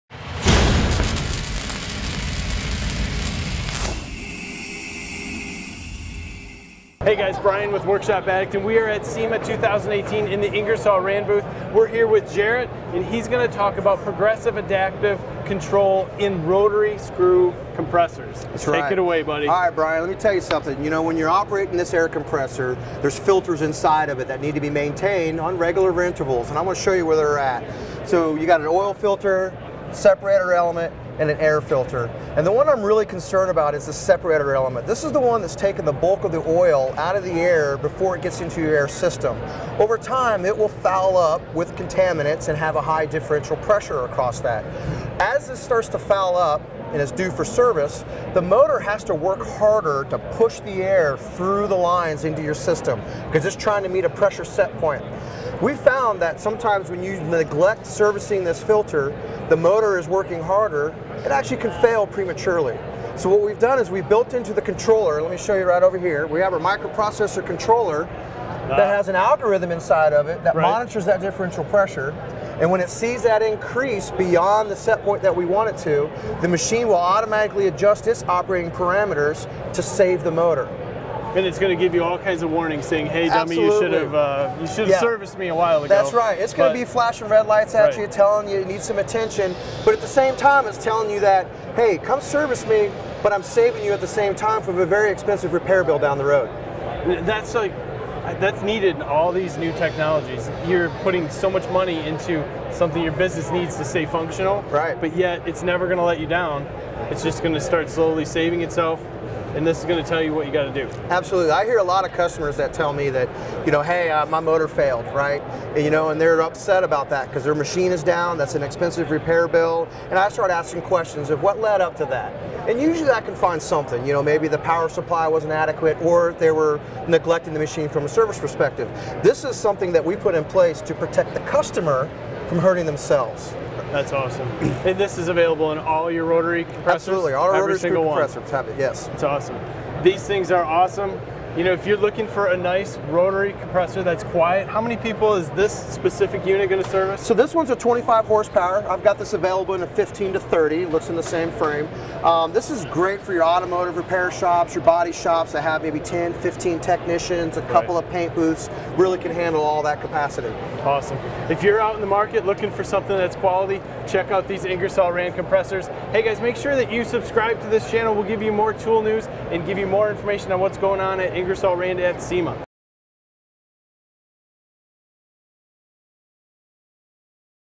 Ingersoll Rand Rotary Screw Compressors Equipped with Advanced Controls to Reduce Downtime
All Ingersoll Rand rotary screw air compressors are equipped with a unique technology called Progressive Adaptive ControlTM. PAC protection is a unique algorithm that monitors key operating parameters and maintenance intervals, and continuously adapts the equipment to prevent motor failures.